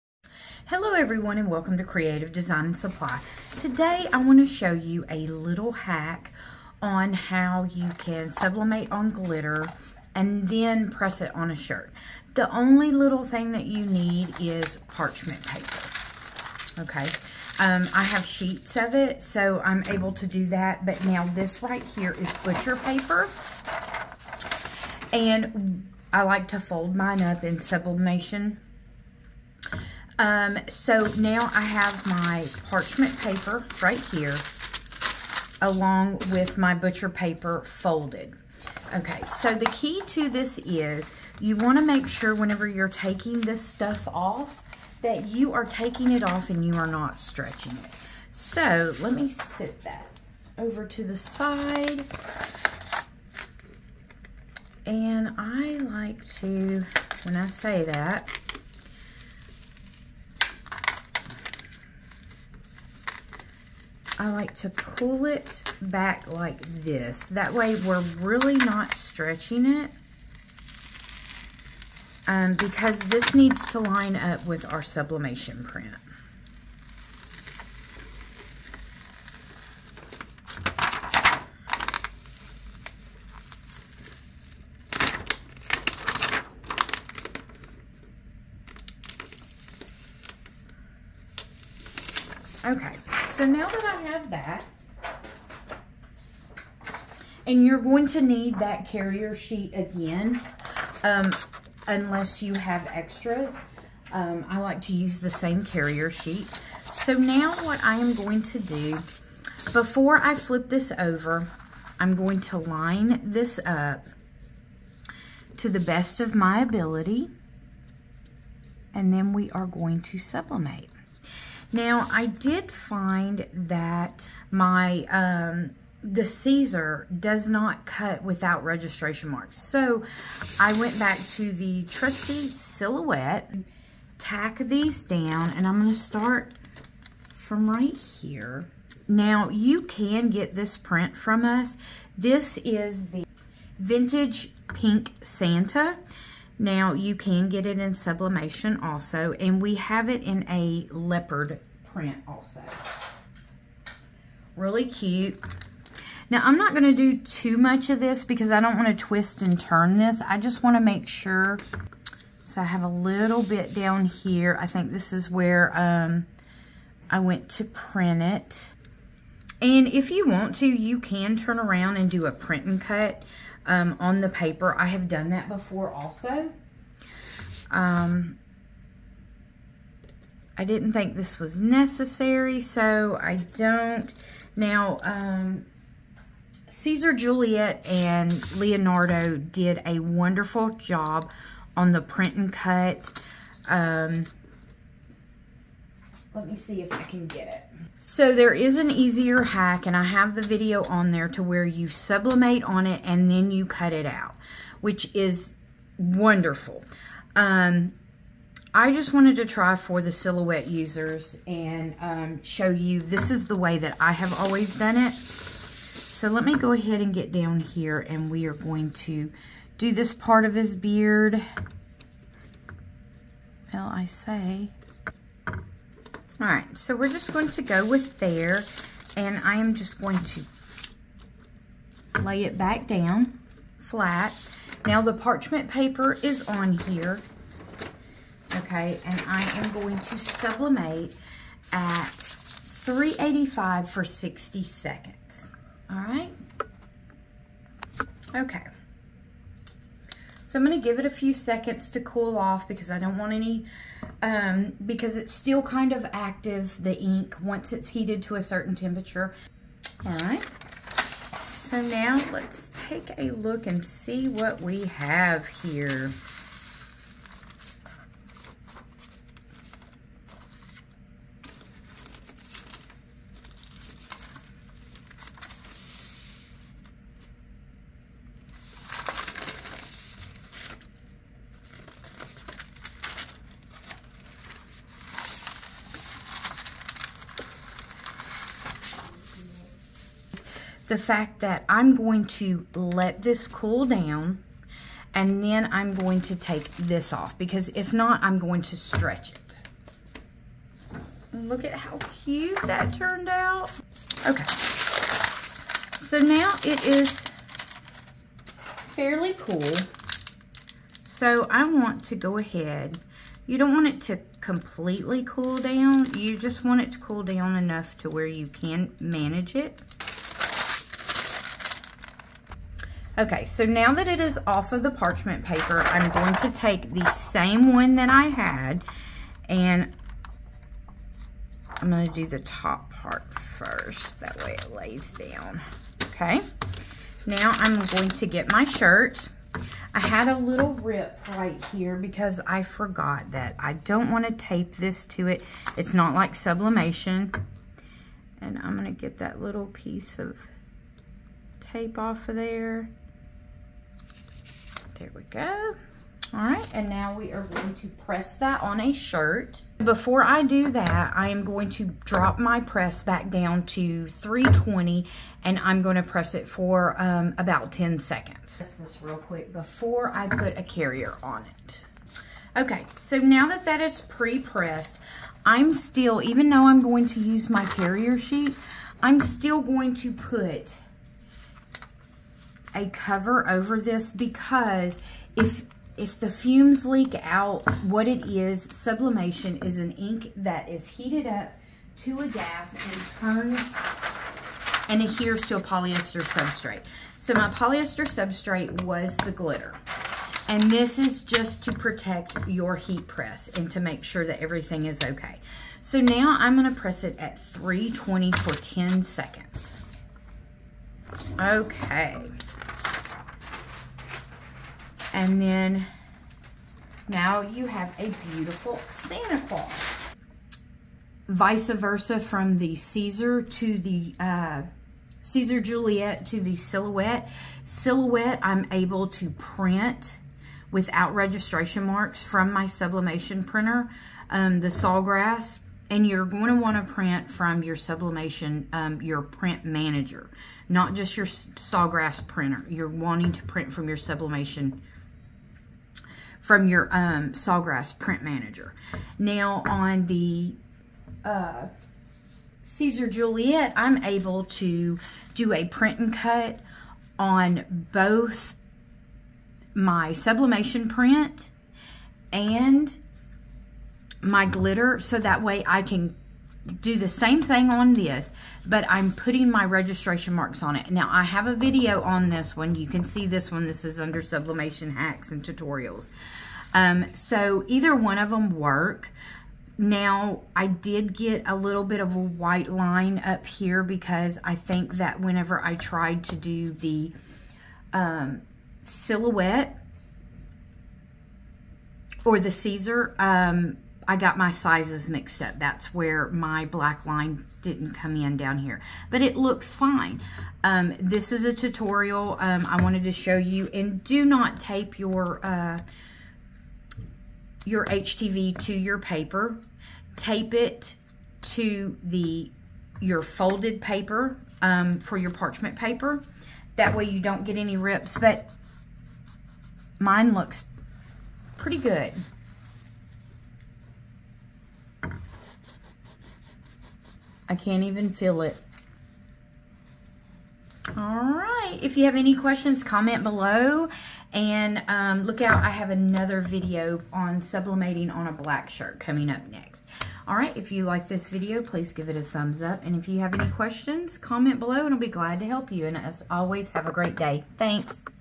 Sublimation on Glitter HTV Before you Press it on a Shirt From Creative Design & Supply
Want to move beyond sublimating white polyester fabrics? You can sublimate on glitter HTV!

Use this trick for adding sublimated images to cotton and dark fabrics. Have you ever wanted to sublimate something other than white polyester fabric? Today I’m sharing one of my favorite tricks for being able to use your sublimated images on cotton and dark fabrics!